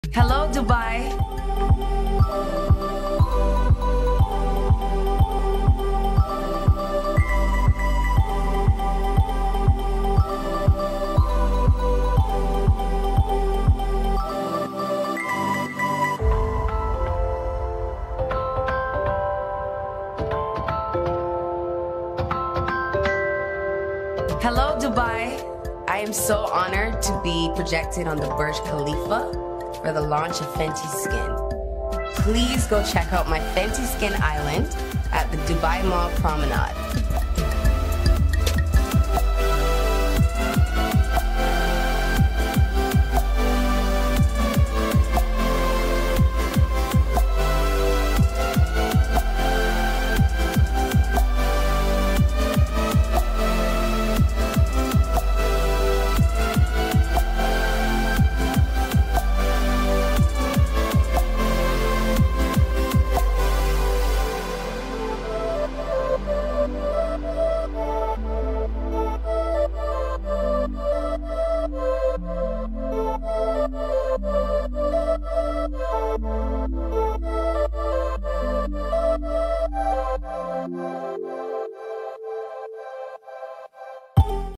ريهانا تظهر على برج خليفة 🥰
احتفلت المغنية الأميركية ريهانا بإطلاق Fenty Skin، خط العناية بالبشرة الخاص بها والذي طال انتظاره من خلال فيديو لها عرض على برج خليفة في دبي 🥰